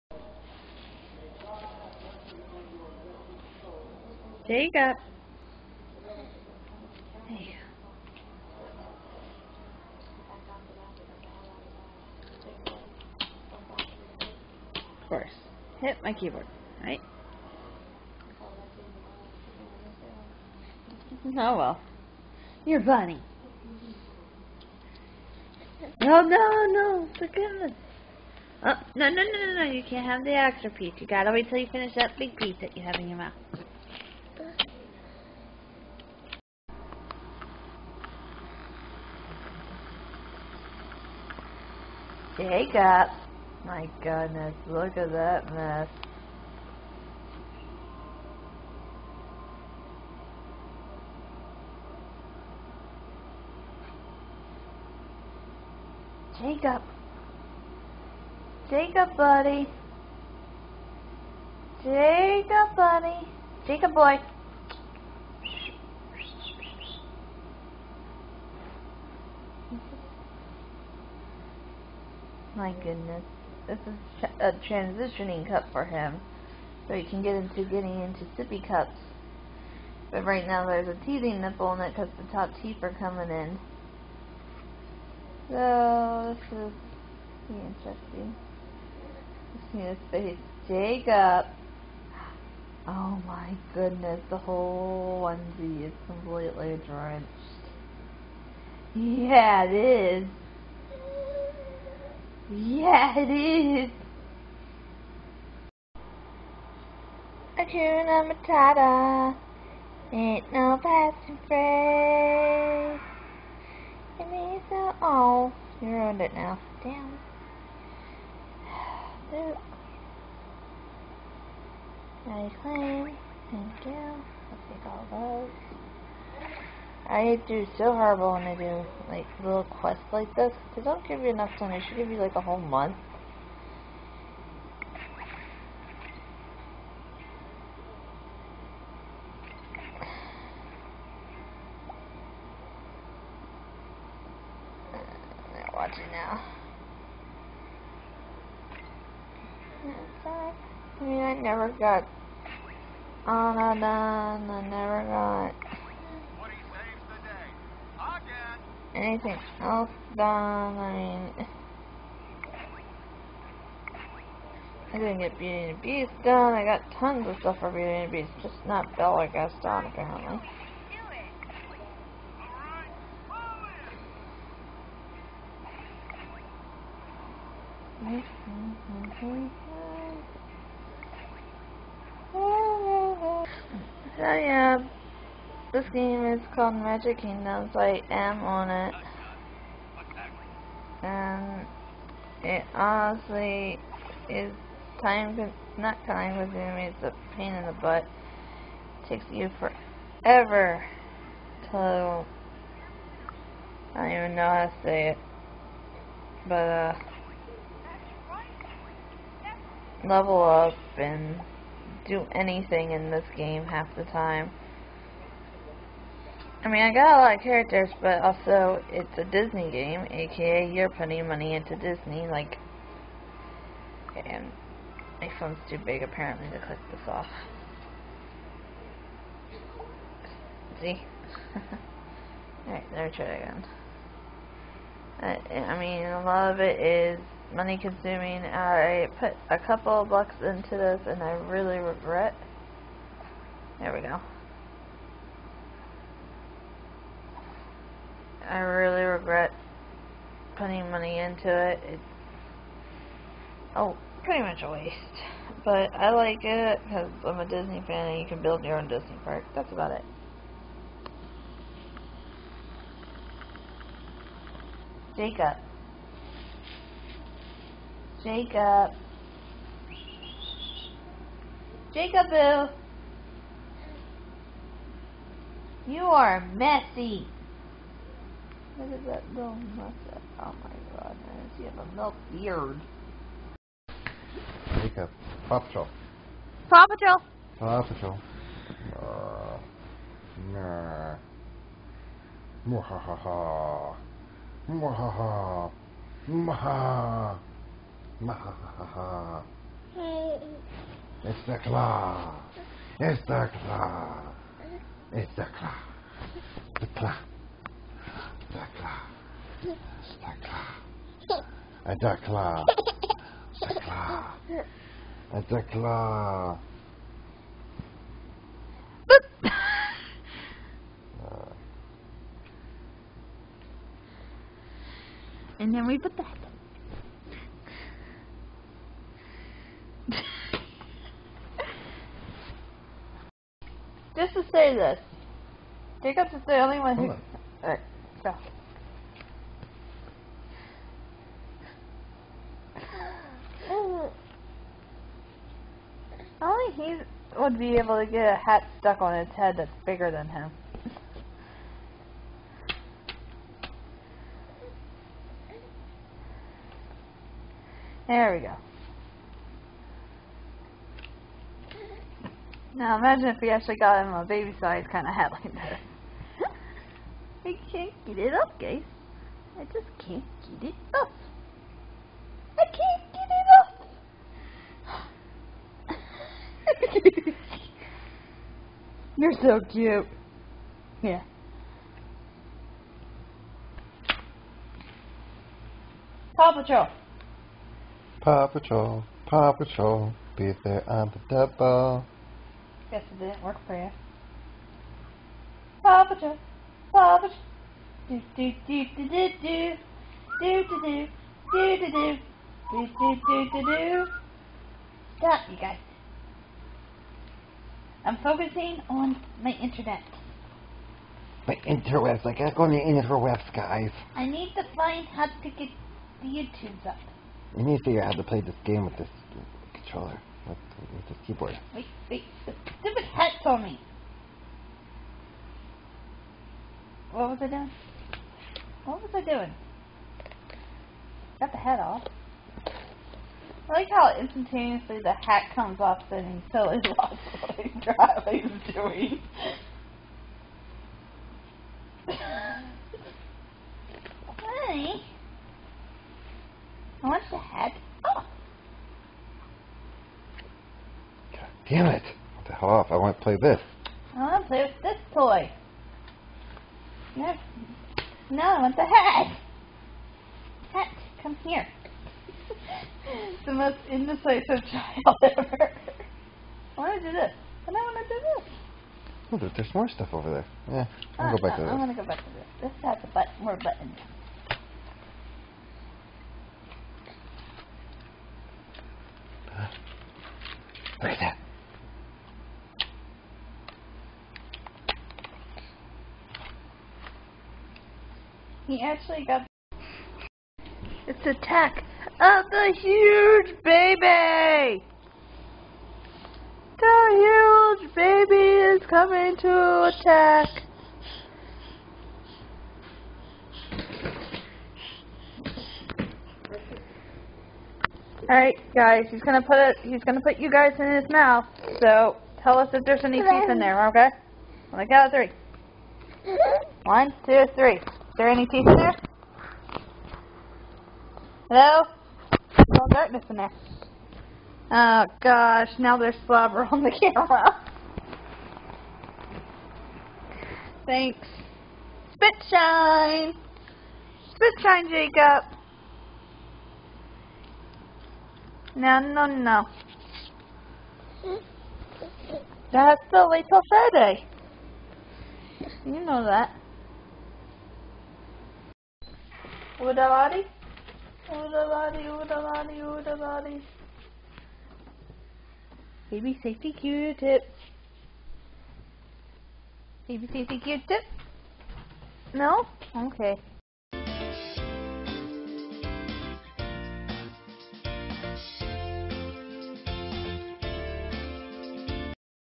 GIANT BABY ATTACK!!! {7.19.17} (664)
today i show you the magic kingdoms app and then the little one is trying to figure things out.